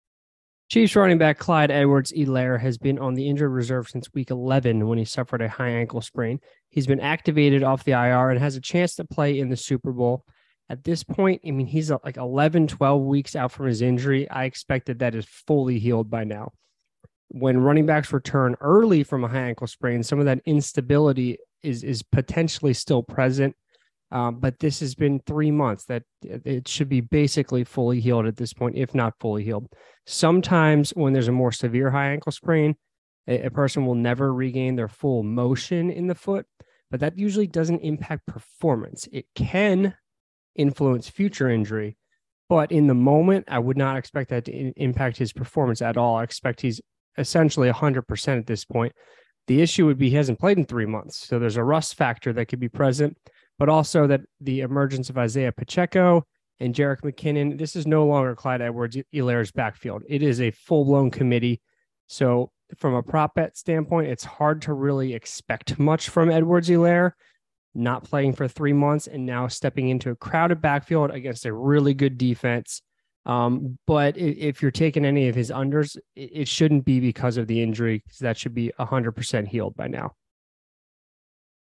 Clyde Edwards-Helaire High Ankle Sprain Super Bowl Preview
Chiefs running back Clyde Edwards-Helaire has been out since week 11 with a high ankle sprain. He has been activated off the IR ahead of the Super Bowl. I discuss if this injury will impact his production.